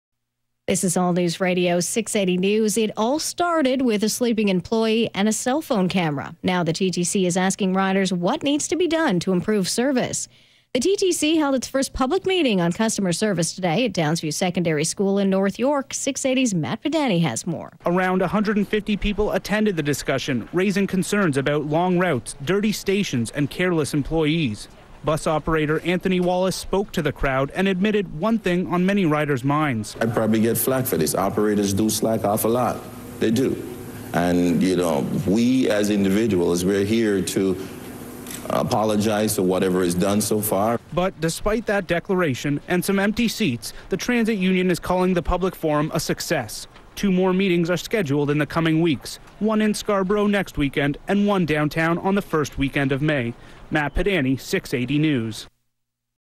Matt Padanyi's radio debut
Matt Padanyi makes his on-air debut at 680News, Canada's largest radio station, on Sunday April 11, 2010... Canadian radio broadcasting history in the making.